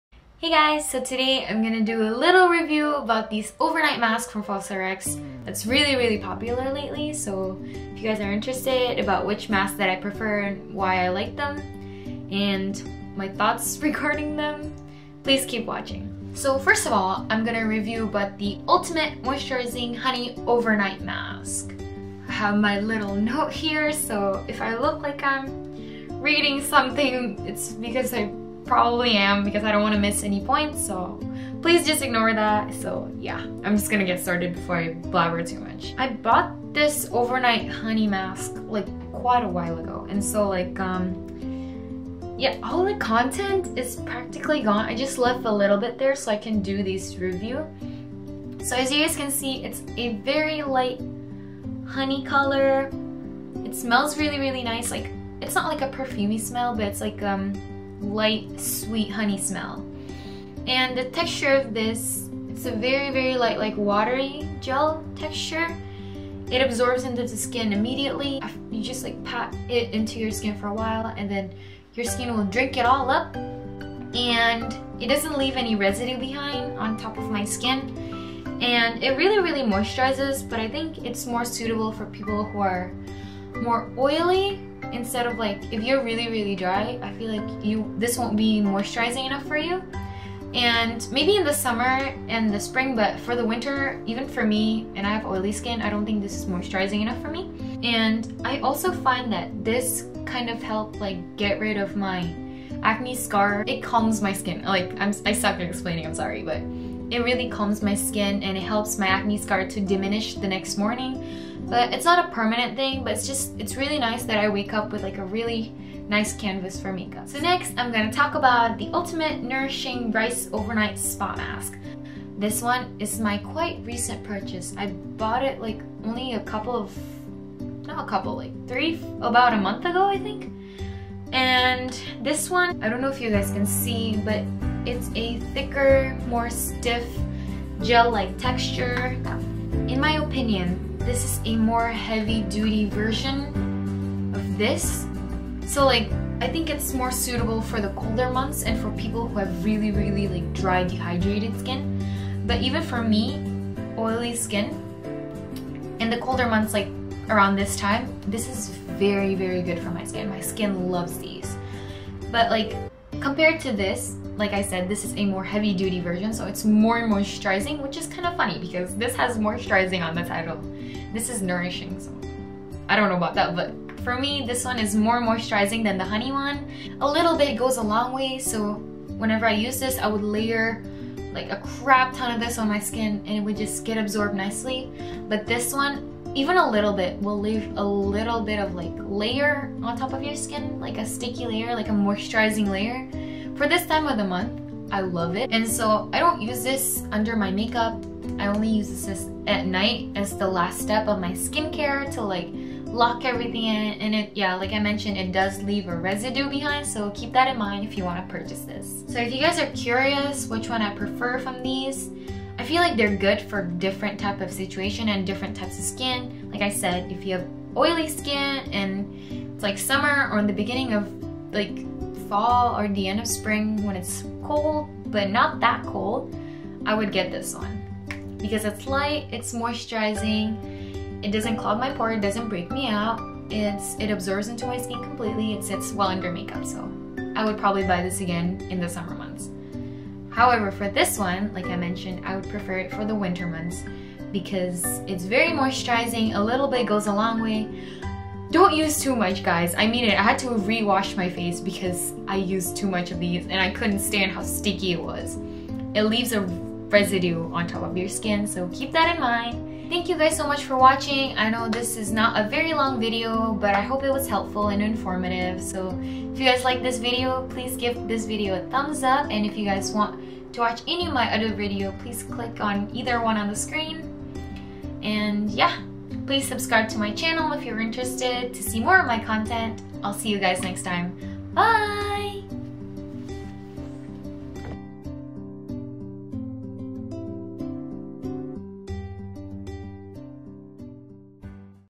Cosrx - Ultimate Honey Overnight Mask & Ultimate Rice Overnight Mask Review (Reupload) | 2016
Hey, guys! This is a short review about the overnight mask that I recently used, I hope it's helpful :D

Products:

COSRX- Ultimate moisturizing honey overnight mask
COSRX- Ultimate nourishing rice overnight spa mask